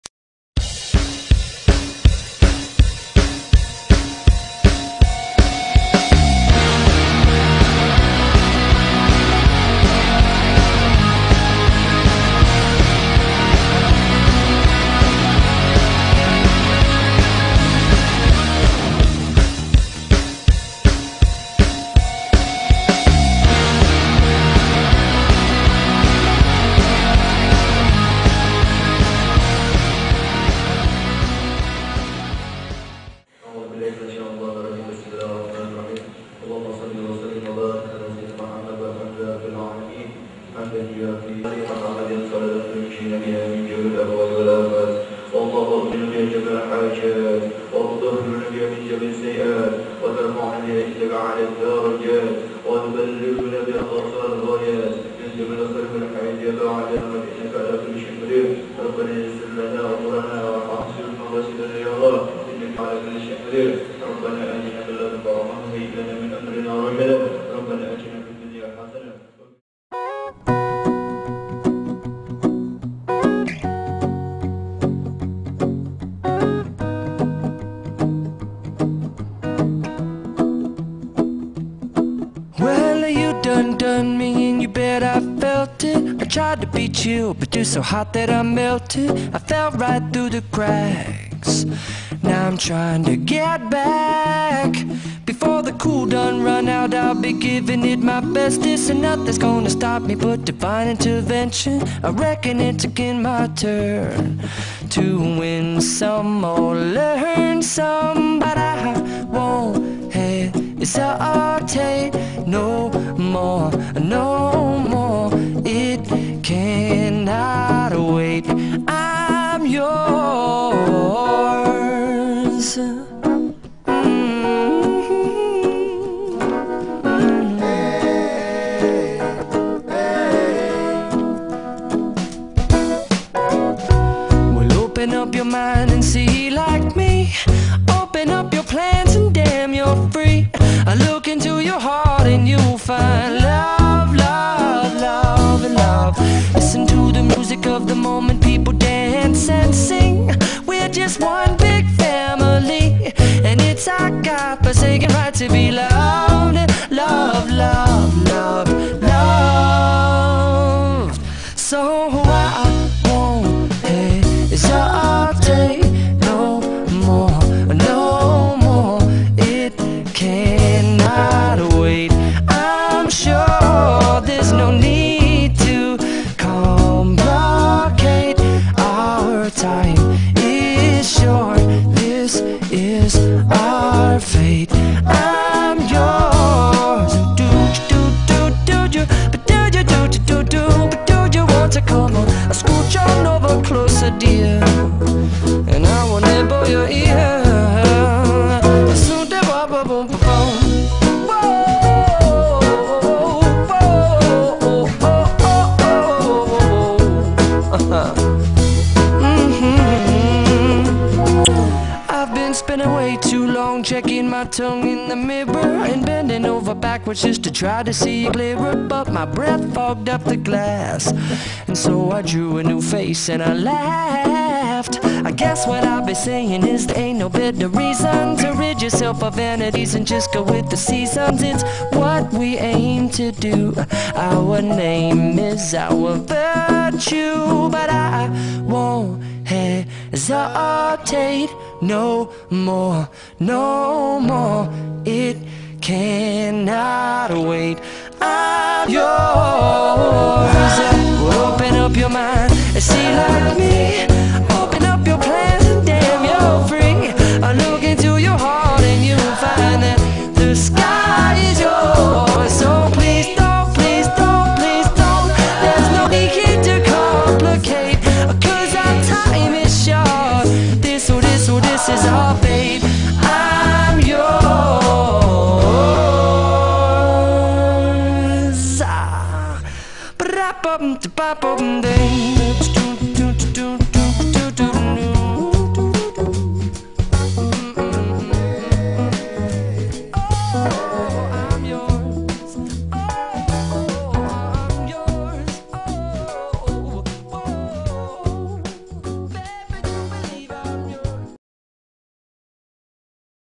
SMP UNTAG SURABAYA (2006-2009) TOUR BALI part 2
Video ini saya persembahan untuk BAPAK DAN IBU GURU SMP UNTAG SURABAYA dan TEMAN TEMAN ALUMNI SMP UNTAG SURABAYA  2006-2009